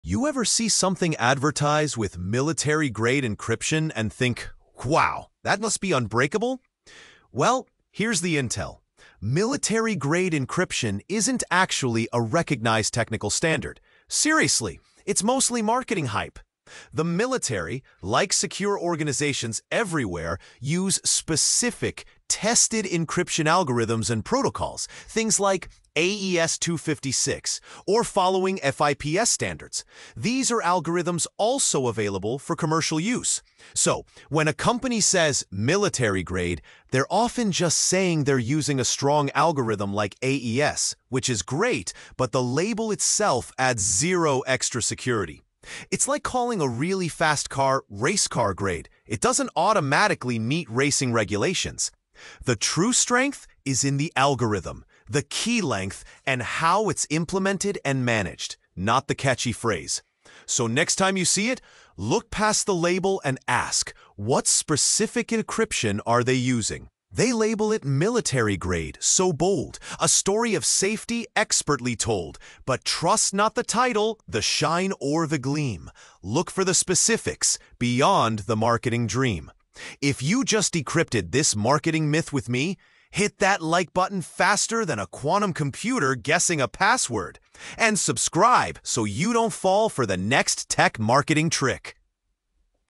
The Truth About "Military-Grade Encryption"
Ever wondered about "military-grade encryption"? 🤔 This YouTube Short reveals why it's mostly marketing hype! Learn what *real* security standards (like AES-256) look like and what to ask for instead of falling for the catchy phrase. It's about algorithms, not labels! Hit like if you decrypted this myth! 👇 cybersecurity encryption tech marketing Check the link for deep dive on this topic